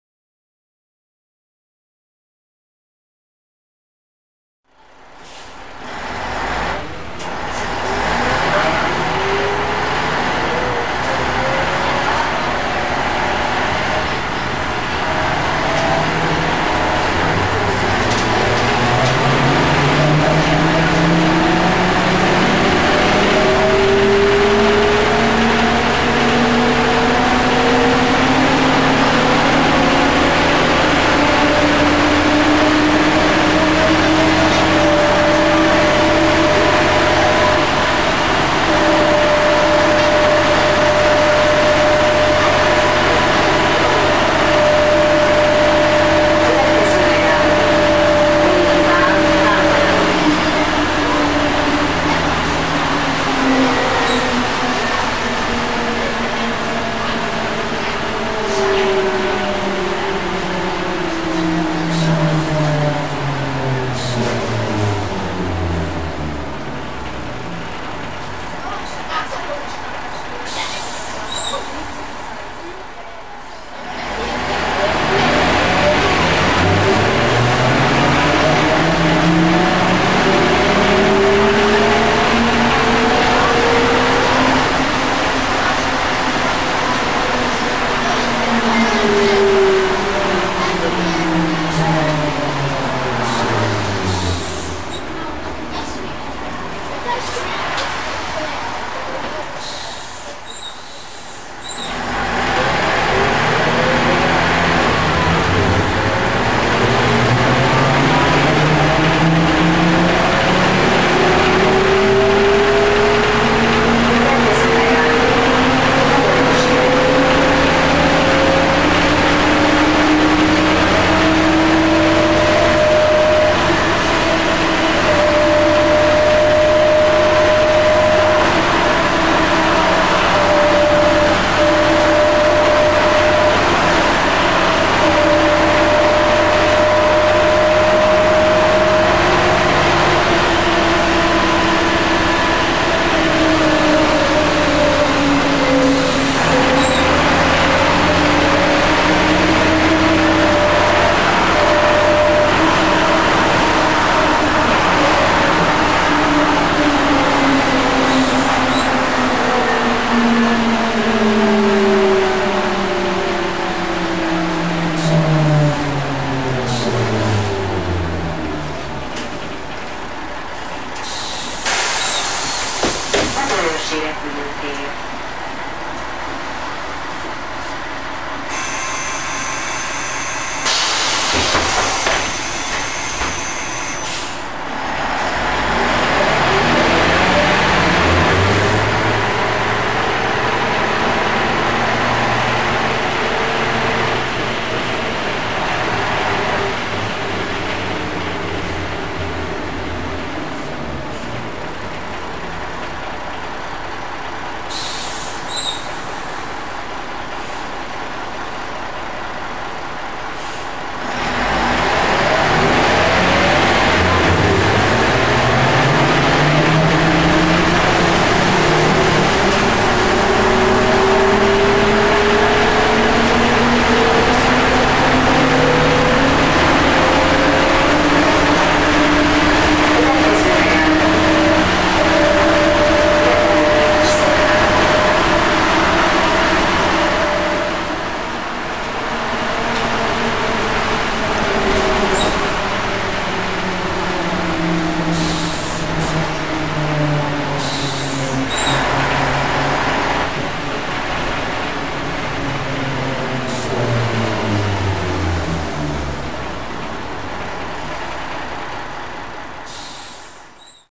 Budapest Bus - Ikarus 280.40A | BPO-457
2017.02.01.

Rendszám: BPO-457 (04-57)
Típus: Ikarus 280.40A

Motor: Rába D10 UTSLL-160 (Euro-2)
Sebességváltó: ZF 4HP 500
Gyártási év: 1987 (vázcserés felújítás: 1998)
Üzemeltető: BKV Zrt.
Telephely: BKV Kőbánya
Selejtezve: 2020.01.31

ex-Gyors 7-173